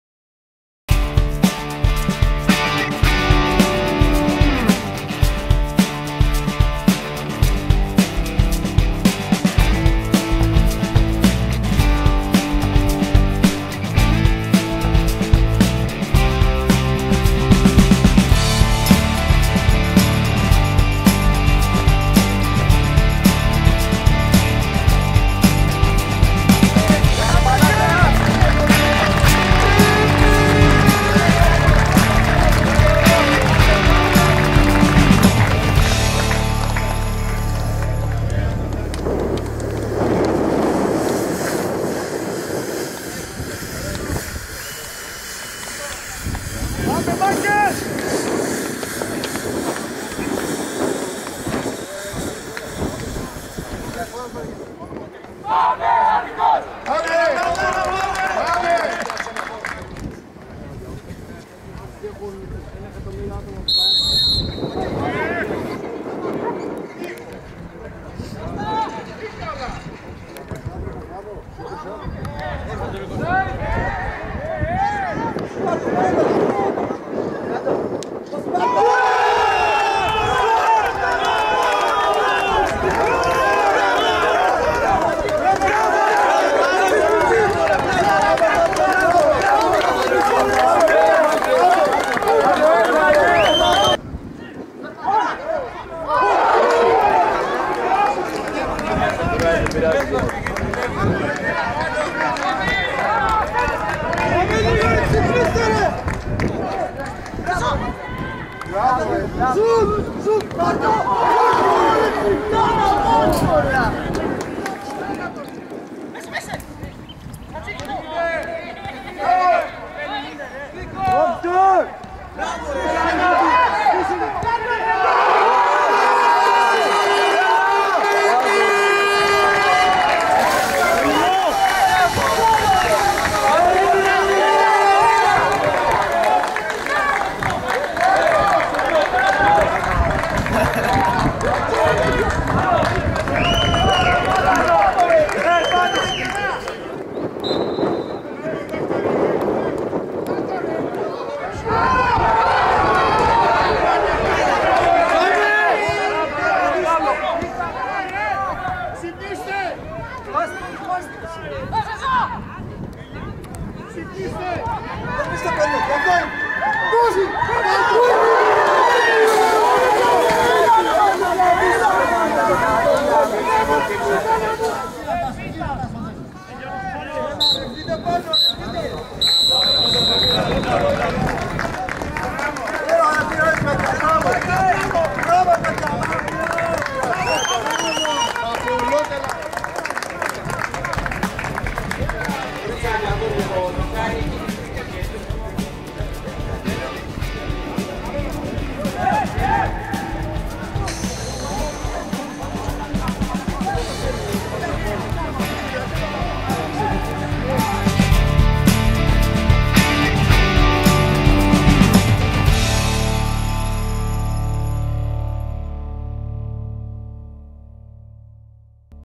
ΠΑΜΠΑΙΑΝΙΚΟΣ-ΠΑΛΑΥΡΕΩΤΙΚΟΣ. Γκόλ - φάσεις
Τα γκόλ και οι φάσεις απο τον σημαντικό αγώνα της Β ΕΠΣΑΝΑ.